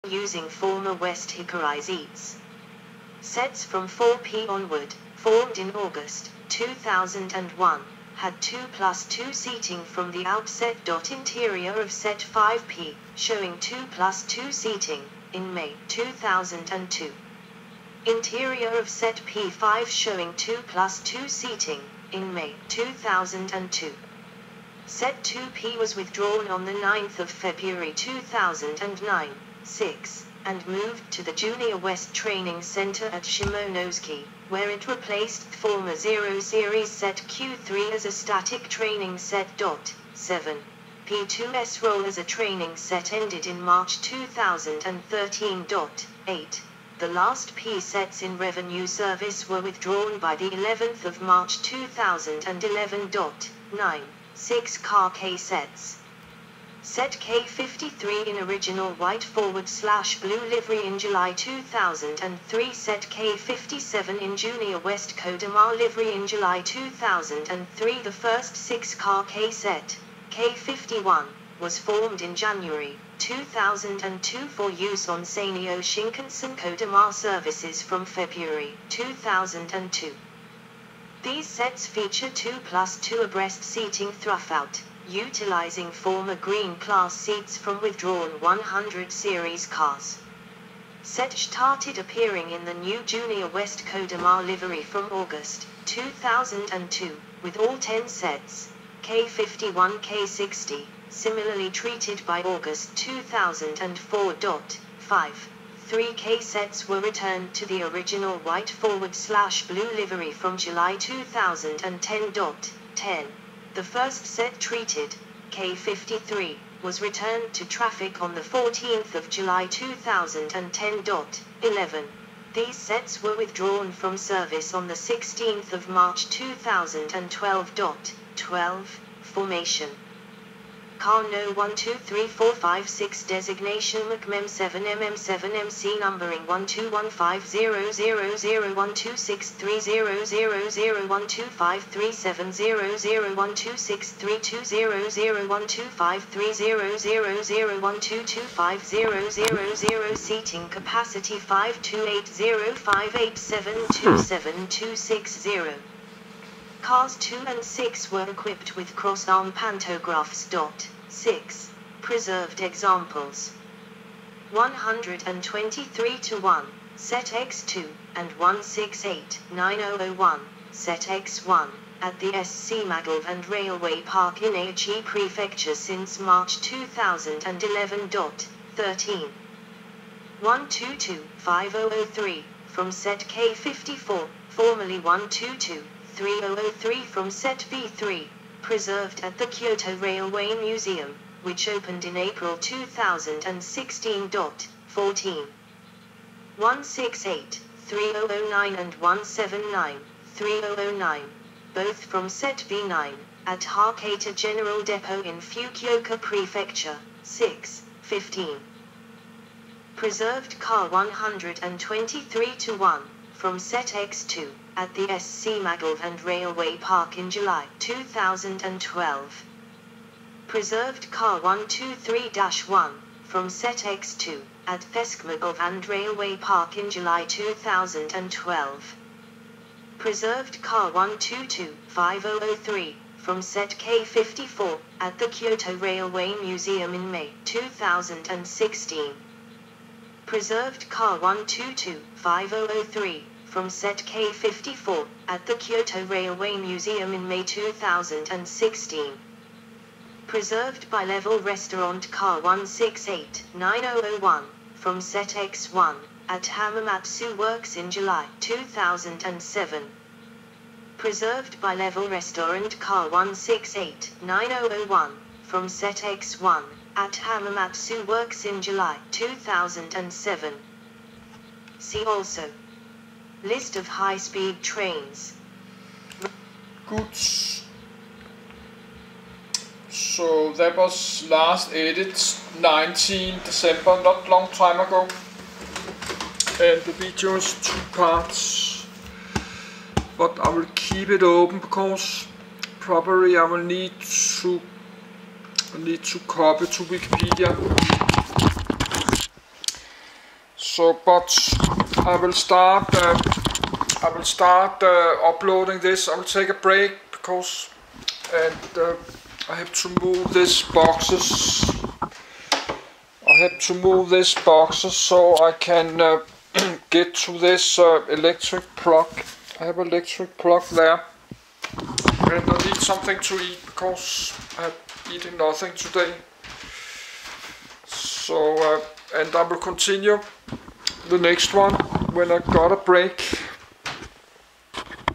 MongoTV_8847 - LOOKING AT GAMER PC HARDWARE - Part 1108 - 100 Series Shinkansen - Japan
PART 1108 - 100 Series Shinkansen - Japan (This video is 2 Part : Part 1107 to Part 1108). They are on my playlist with name "MONGO TV - LOOKING AT GAMER PC HARDWARE") I like try do this, to get help read difficult technical English text loud. That will help me a bit to understand the text :-) Maybe this video could be helpful for some other too, that don't speak and read English very well :-) Have a good day too.